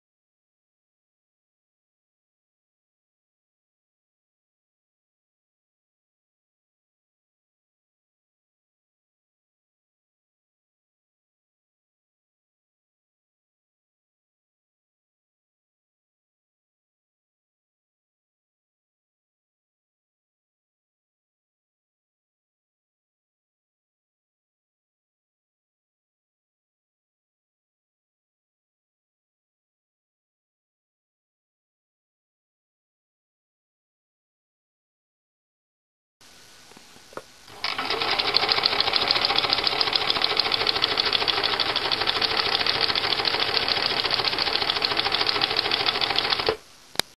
Husqvarna Viking Walking foot P60444
Using a standard walking foot on a Husqvarna Viking sewing machine. P60444